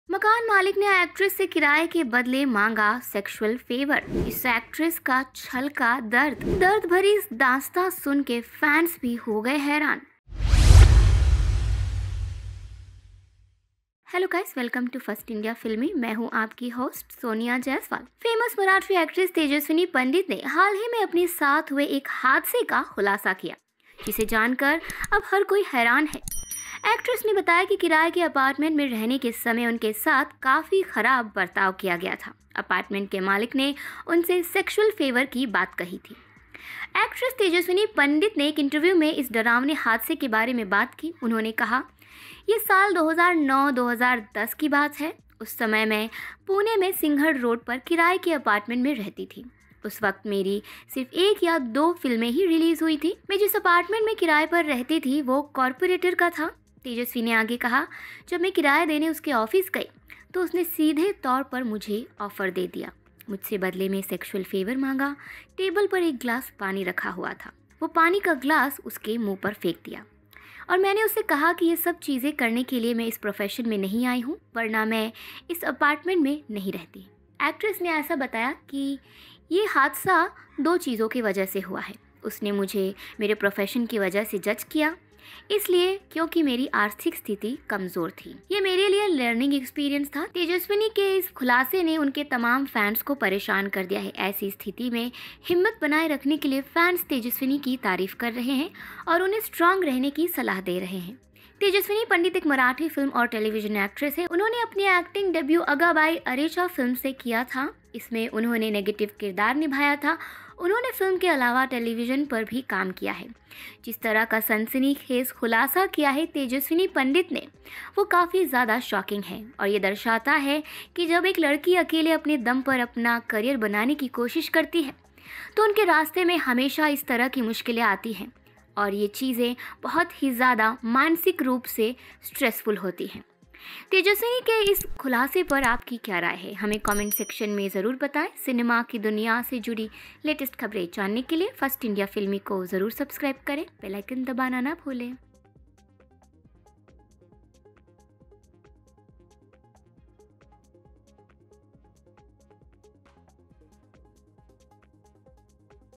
Tejaswini Pandit के सनसनीखेज़ खुलासे ने किया Fans को हैरान, आर्थिक तंगी की सुनिये दर्दनाक दास्तान
Marathi actress Tejaswini Pandit recently opened up about a humiliating experience she had while staying at a rental apartment in Pune years ago when she was struggling to come up the ranks.

Reporter: Sonia Jaiswal
Editor: Tushar Shukla